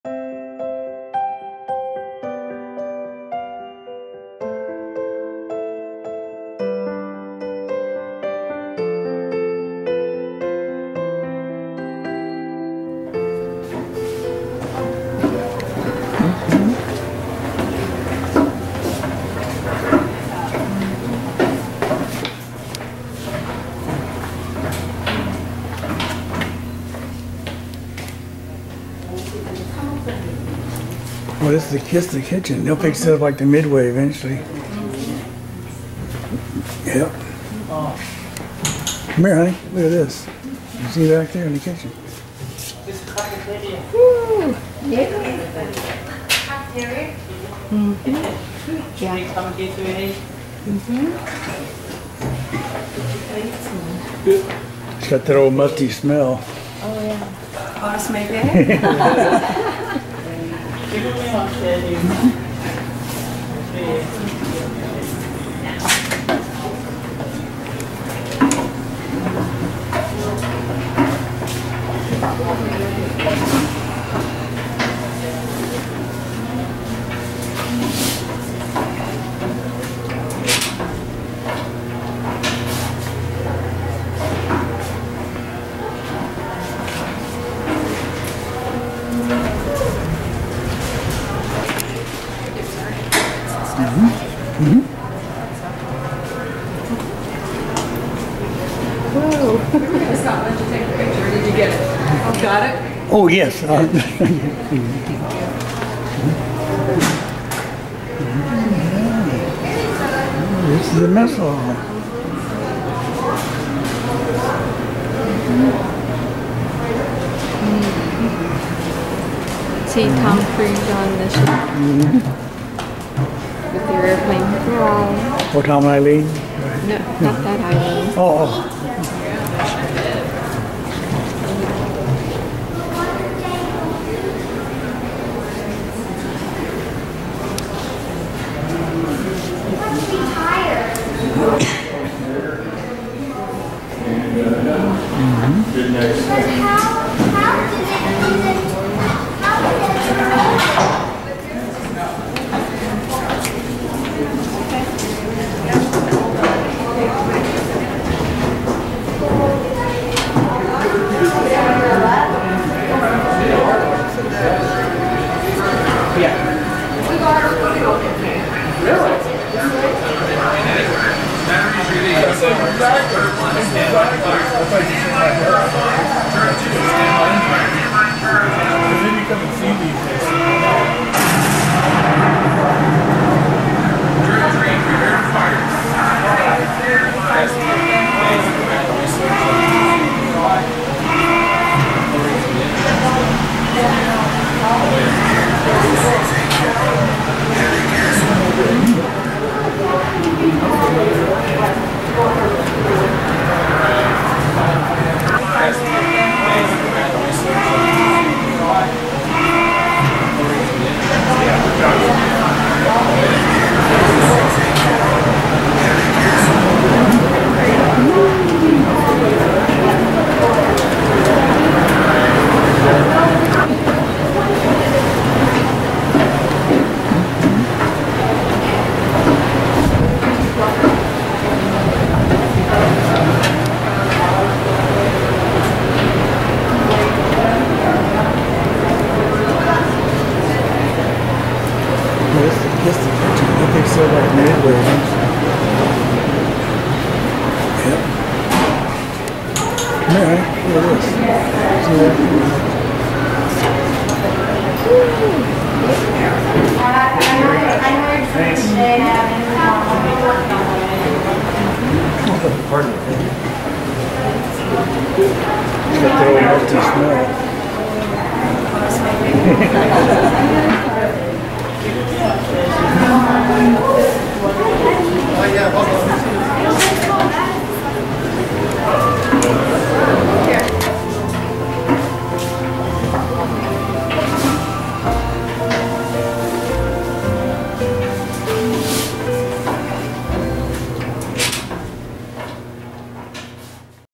The USS IOWA Battleship
Exploring below deck on the USS IOWA Battleship in San Pedro California.